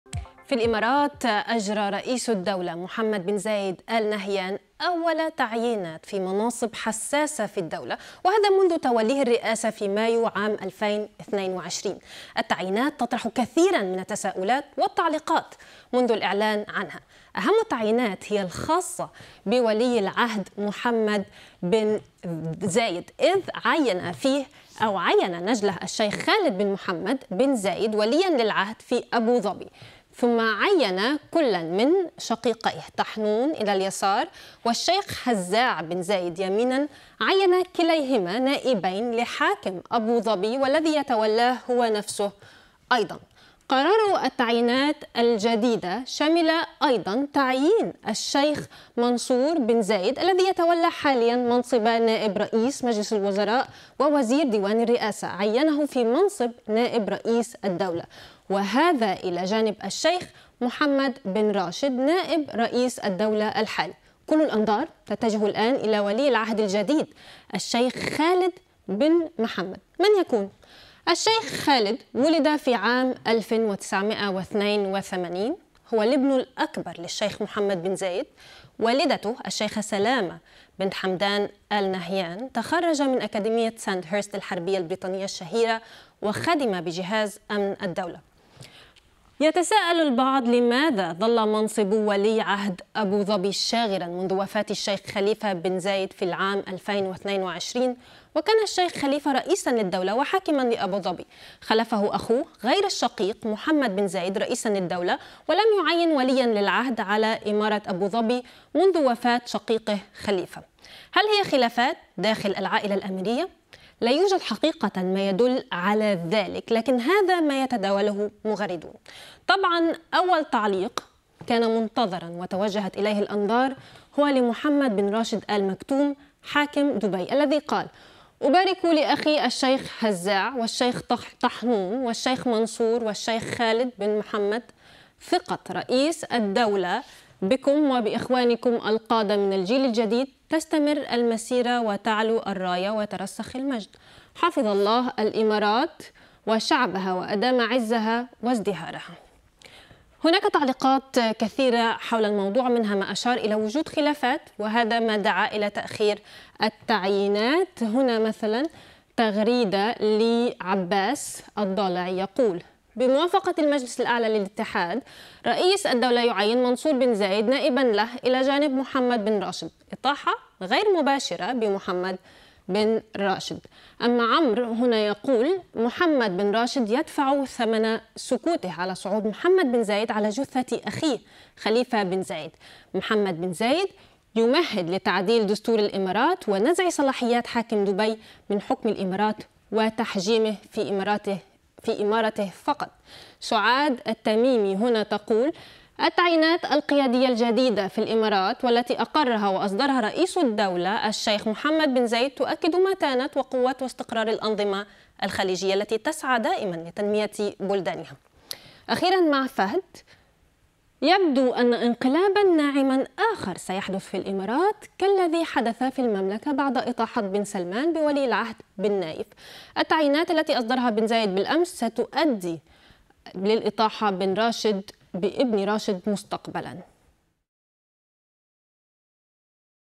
ماذا يعني تعيين رئيس الإمارات محمد بن زايد نجله وليا للعهد لإمارة أبو ظبي؟
عيّن رئيس دولة الإمارات الشيخ محمد بن زايد آل نهيان نجله الشيخ خالد بن محمد بن زايد وليّاً للعهد في إمارة أبوظبي، كما عيّن إخوته في مناصب عليا أخرى.
وتعتبر التعيينات الجديدة الأولى من نوعها منذ تسلمه منصبه في أيار/مايو 2022 إثر وفاة شقيقه الشيخ خليفة بن زايد آل نهيان.